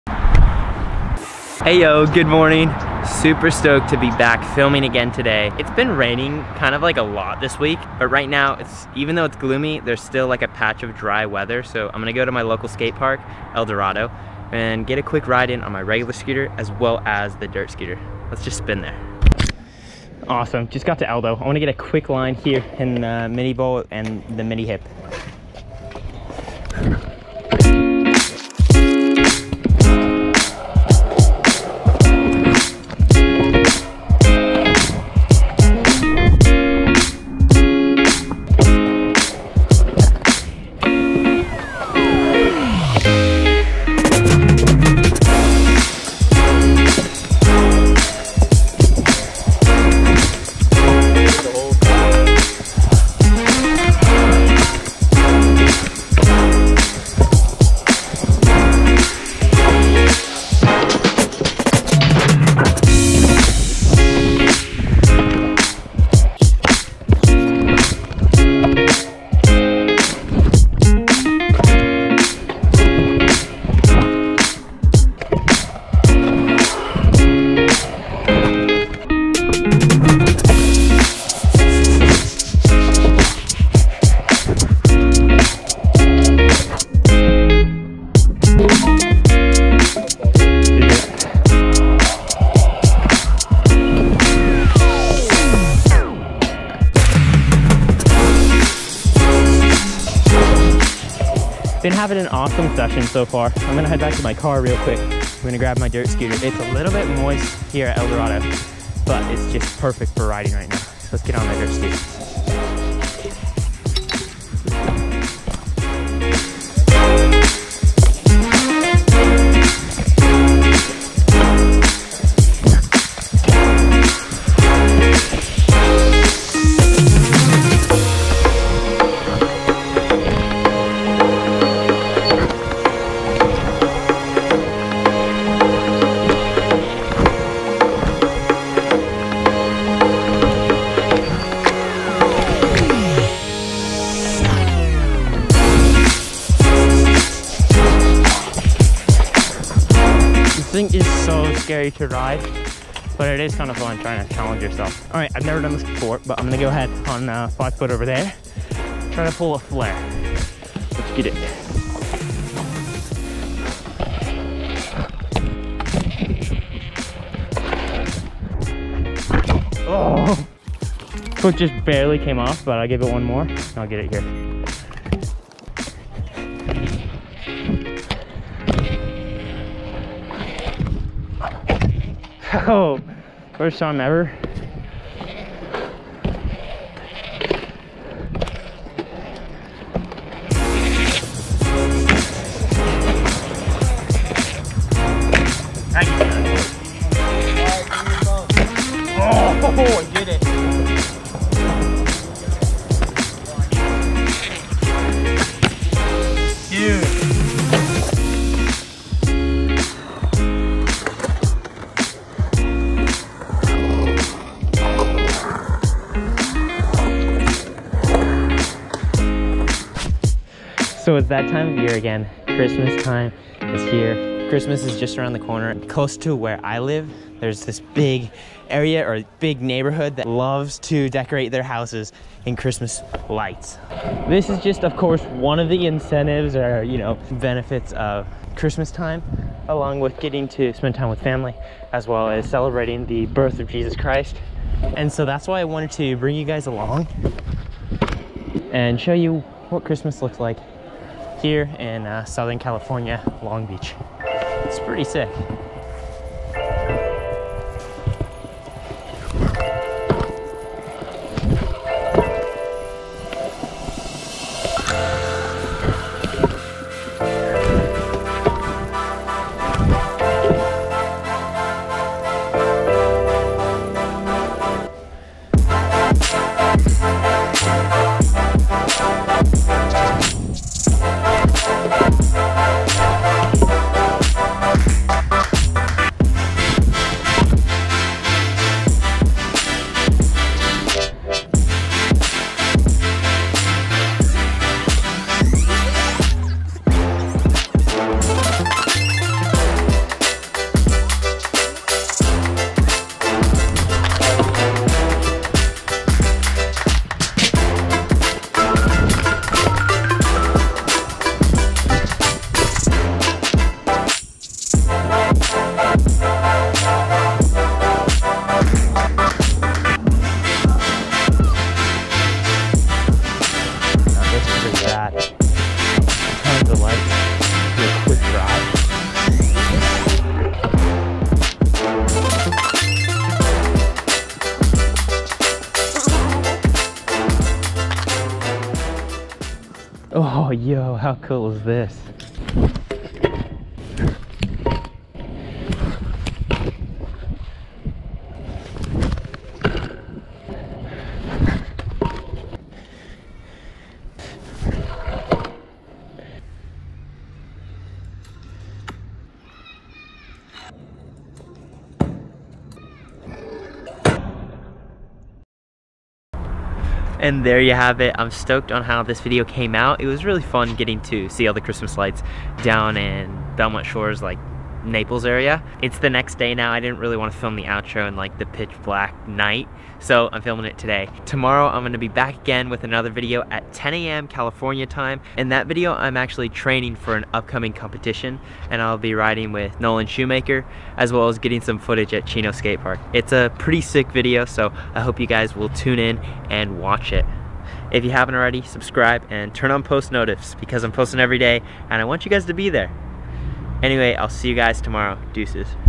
Dirt scooter flair at the skatepark!
Check out my last video where I rated the best skateparks in LA:

Super fun getting to land my first flair on the dirt scooter, and also getting to look at some christmas lights in this video. Christmas is the best because you get to spend time with people you love and you also get to celebrate the birth of Jesus Christ!